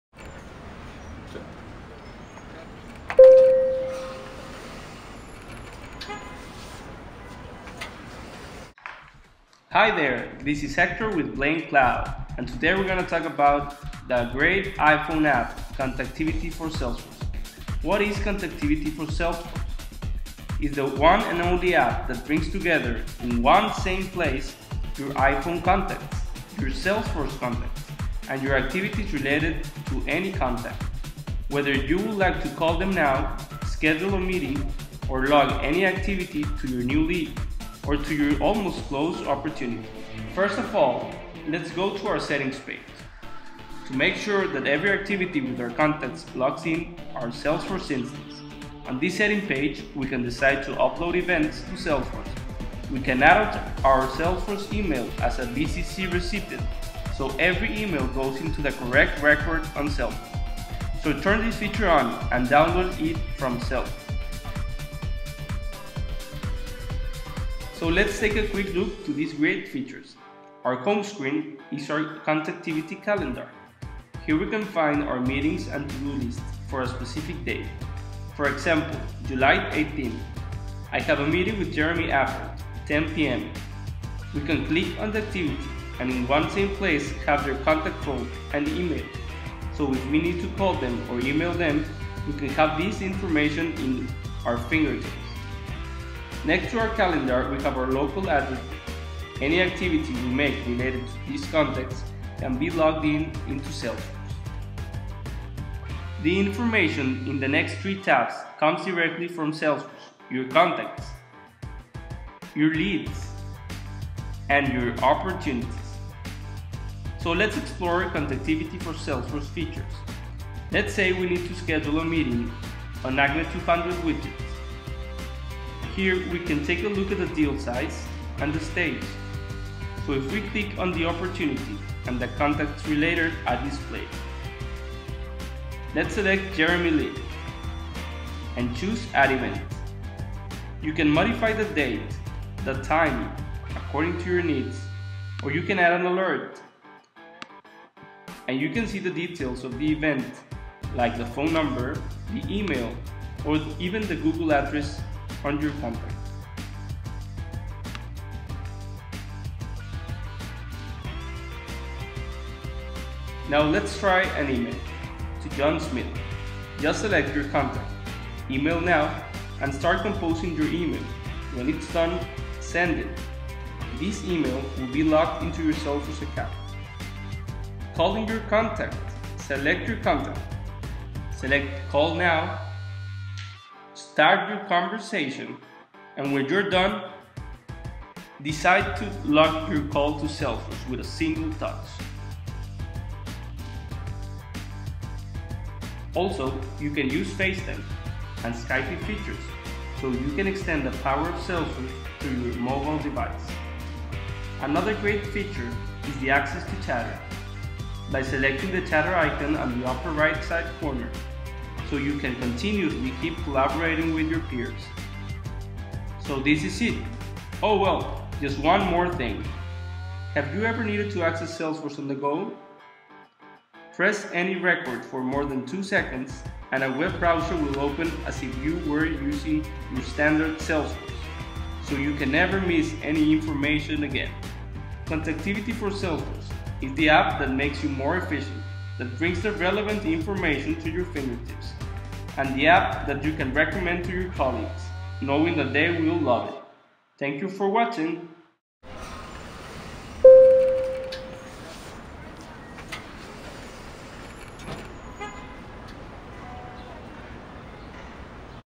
Contactivity for Salesforce - Download it from the Appexchange or App Store
Contactivity for Salesforce is the easiest way to manage your Contacts and Activities in one same place, is the best app to extend the power of Salesforce.com. Increases Productivity by working in a different way, it's fast, it's productive, it's Cloud.

"Unleash the power of your iPhone to Drive Sales Productivity with
Salesforce"

Features included (What can I do with Contactivity for Salesforce):
- Easily and faster, create events in your iPhone/iPod, they will be
uploaded to your Salesforce.com ORG.
- After your call decide with a single click if you want to log this in
the activity history of leads, opportunities and contacts.
- Send emails from your iPhone/iPod and log them in the activity history.
- We've added FaceTime and Skype functionality so you can keep track of every call and never miss logging them.
- Need to access your Salesforce for more information?, with just one click access your complete ORG.
- On the Go, share your dashboards screenshots or any attached file.
- With Contactivity for Salesforce manage your iPhone/iPod Address Book and Salesforce Contacts in one same place with great functionalities to increase Productivity.

Additional features:
- Manage multi-currency opportunities.
- One click access to chatter.
- No information is stored in the device to enhance data security.
- Full functionality for Salesforce.com licenses that supports API calls.